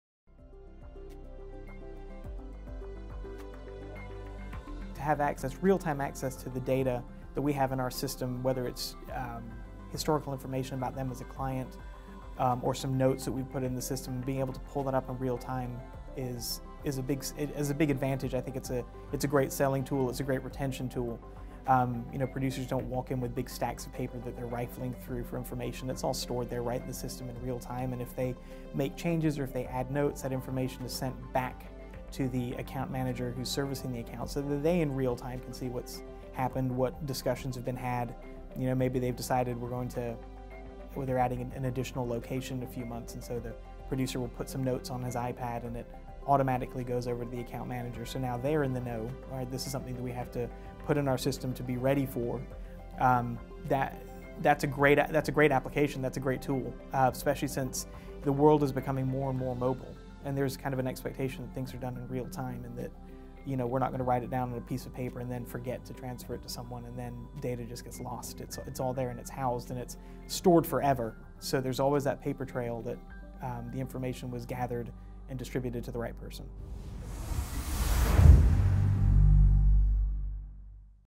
Pritchard & Jerden - Mobile
Through the connectivity of Applied technology, Pritchard & Jerden drives business growth by easily connecting to its insureds and insurers. Watch this video and see how Pritchard & Jerden leverages Applied technology to thrive as a digital agency.

"Applied is our partner in creating a full digital agency. We can see that with the insurer connectivity, insured connectivity, the producer mobile app, as well as hosting our agency management system in the cloud."
- Jim Bailey, President